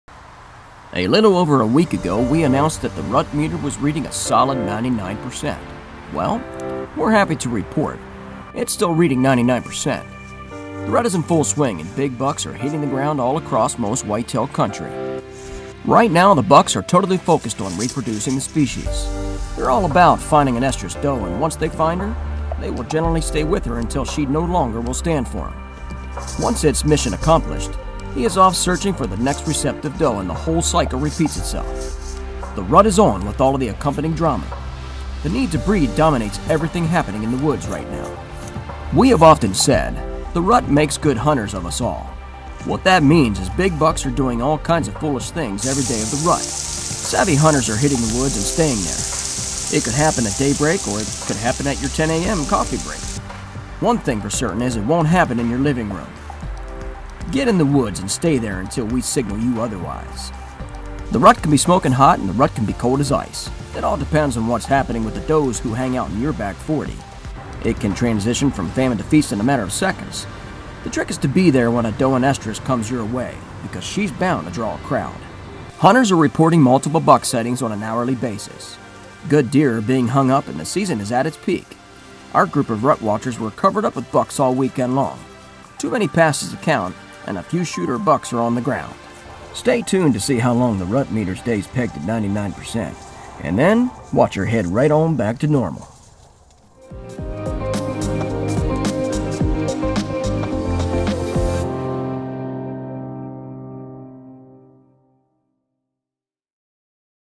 The Hunting Page's Rut Tracker 2017 - Episode 9
Semi-Live Rut Tracker written by Craig Dougherty and filmed with Stealth Cam DS4K Trail Cameras!